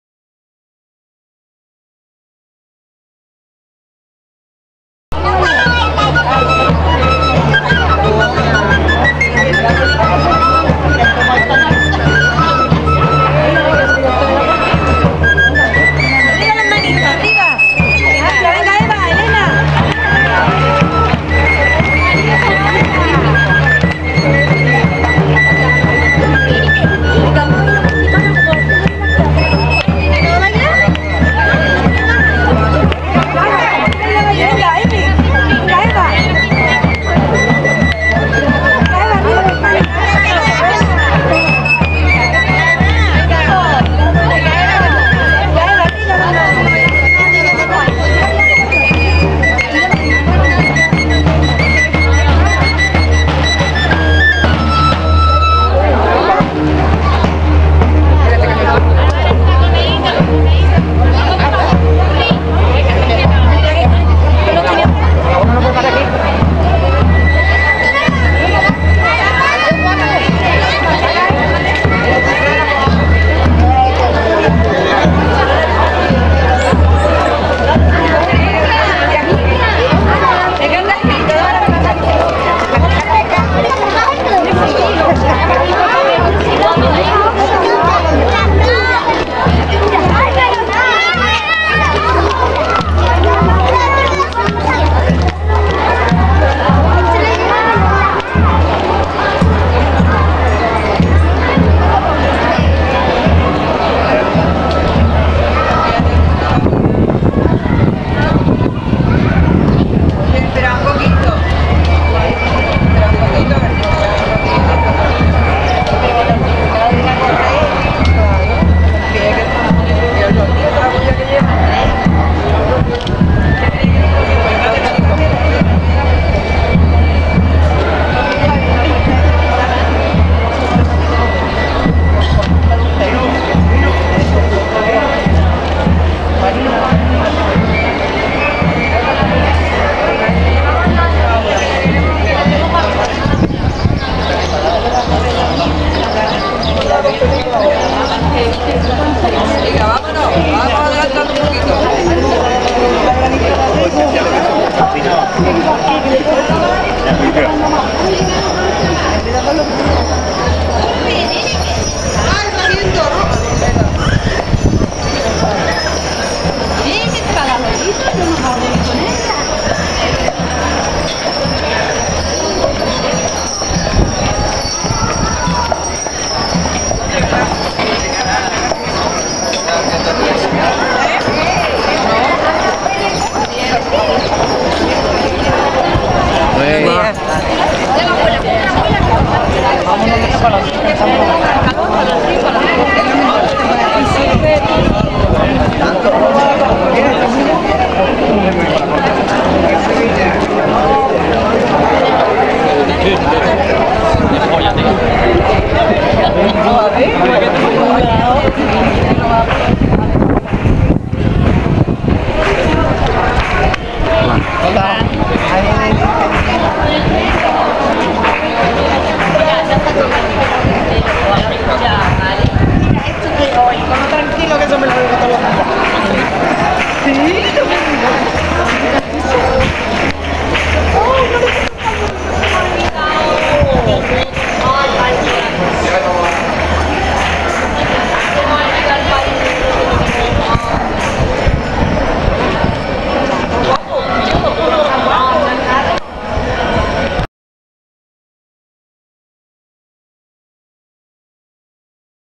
SALIDA DE LOS ROCIEROS DE ARCOS HACIA LA ROMERÍA DEL ROCÍO 2011
SALIDA DESDE LA PARROQUIA DE SAN FRANCISCO DE ARCOS